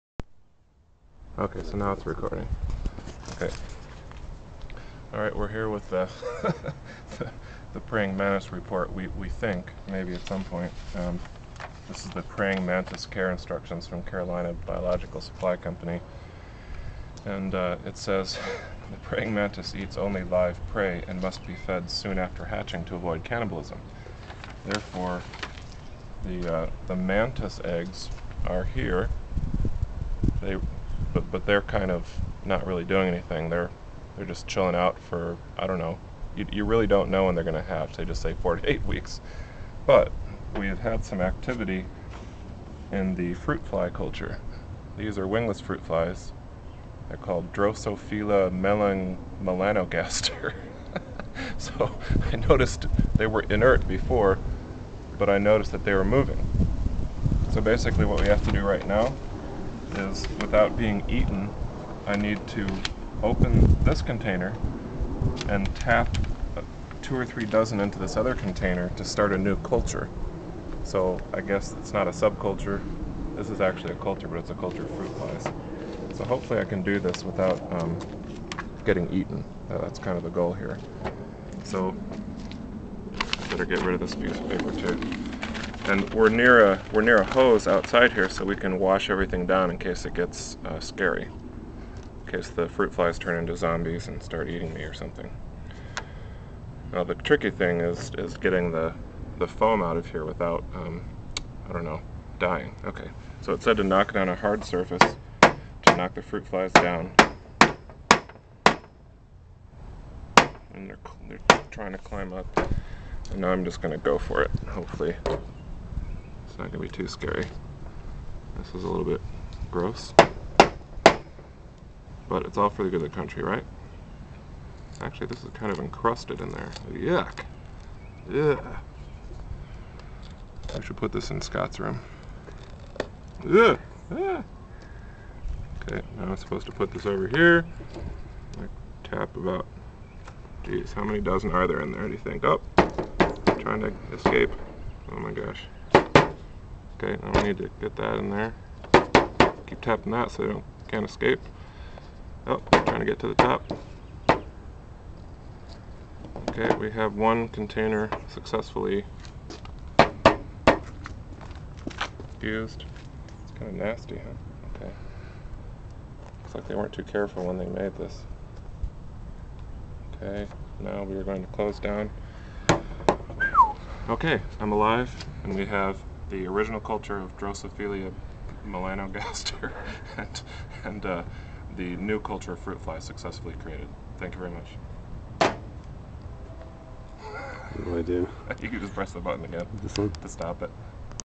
Dr. Fun - Mantis Report - Preparing Fruit Flies
When the fruit flies arrived in the mail, I had to go through the process of transferring them and starting a new culture. trying to raise praying mantises.